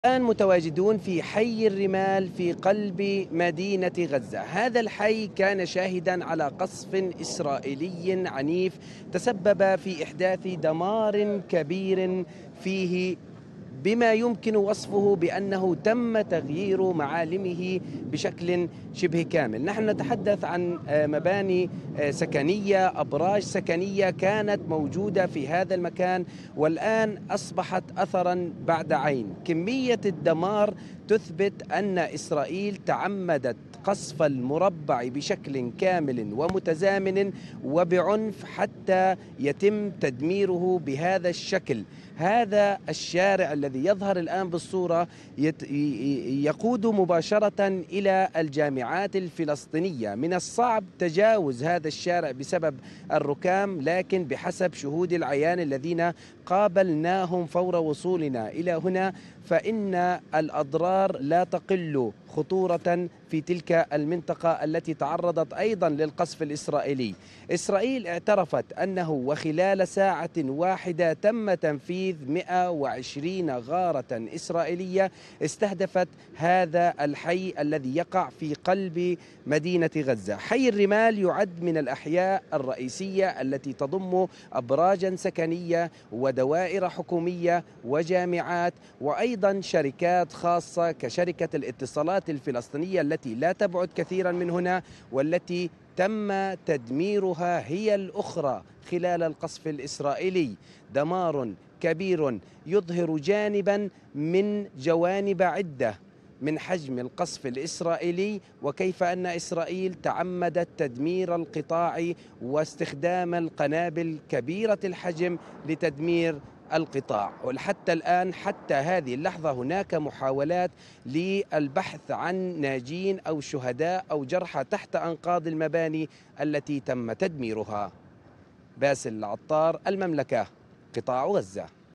حي الرمال قلب مدينة غزة.. طائرات الاحتلال الإسرائيلي تغير معالمه
طائرات الاحتلال الإسرائيلي تغير معالم  حي الرمال في قطاع غزة

مراسل المملكة باسل العطار

ـــــــــــــــــــــــــــــــــــــــــــــــــــــــــ
ـــــــــــــــــــــــــــــــــــــــــــــــــــــــــ
قناة المملكة
المملكة
AlMamlakaTV
Al Mamlaka
أخبار الأردن
أخبار العالم
ـــــــــــــــــــــــــــــــــــــــــــــــــــــــــ
قناة المملكة